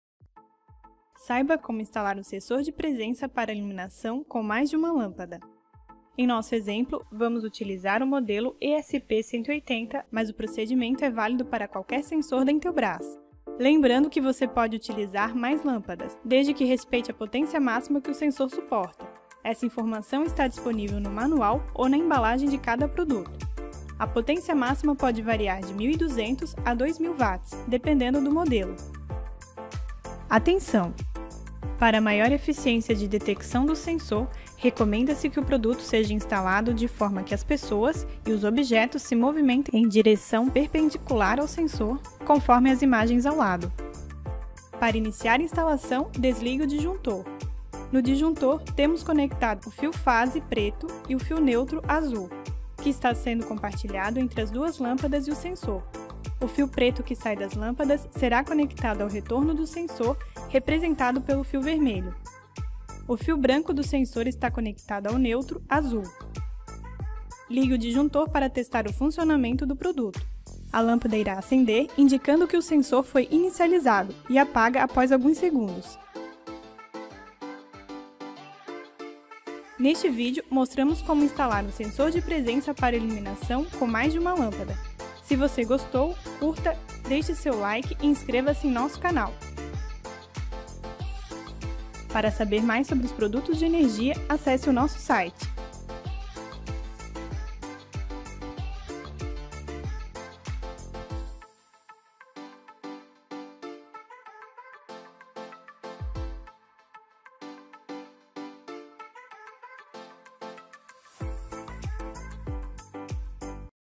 Saiba como instalar um sensor de presença para iluminação com mais de uma lâmpada - i1809
Neste vídeo, vamos mostrar a instalação de um sensor de presença para iluminação para acionar mais de uma lâmpada.